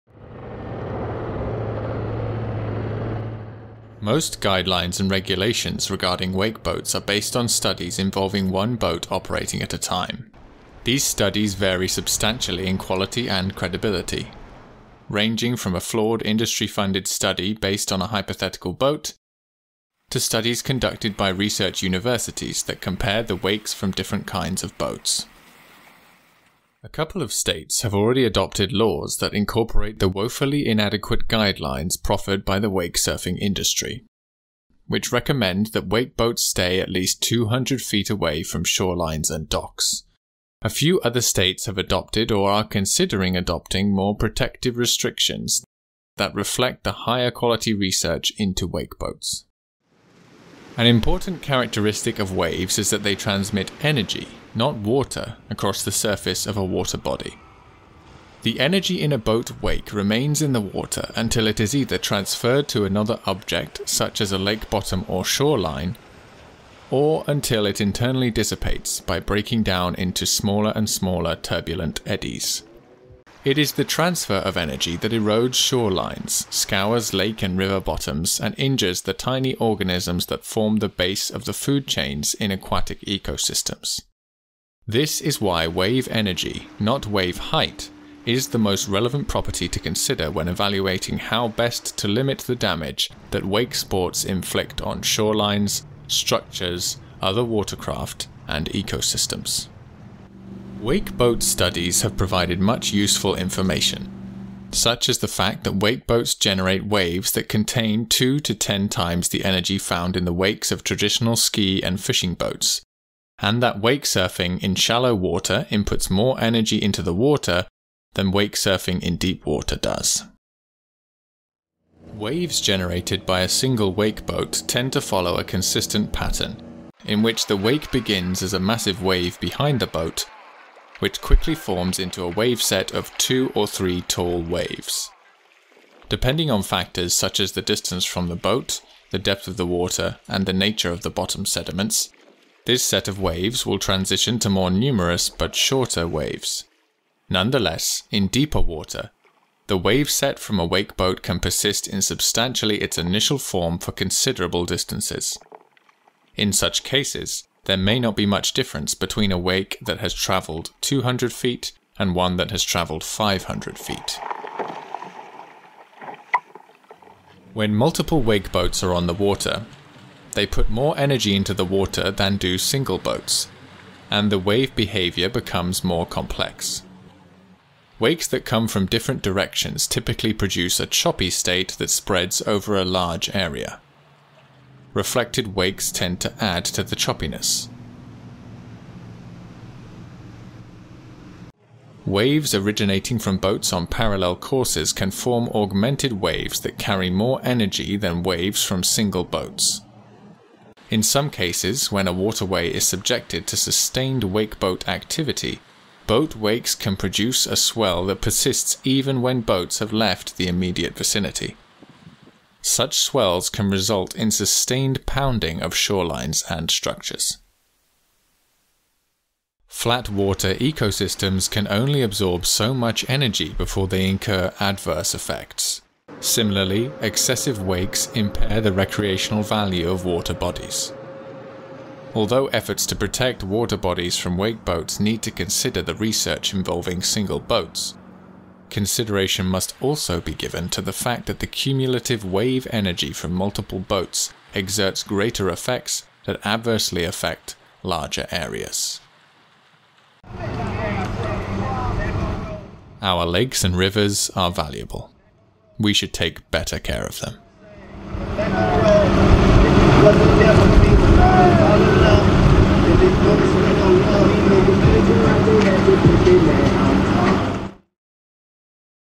The Multiple Wake Boat Problem
Recent and ongoing research regarding wake boats is providing useful quantitative information regarding important parameters such as wave energy. Nonetheless, it is important to be mindful that this research involves the effects of single wake boats. Because it is common to have several wake boats engaged in wake sports at a time, additional allowances for the increased energy inputs should be taken into account when fashioning measures to protect aquatic environments, shorelines, and other boaters from the damage caused by these problematic boats..

For additional information regarding the problems associated with wakesurfing, wakeboarding, and tubing with wake boats, watch the other videos on my YouTube channel.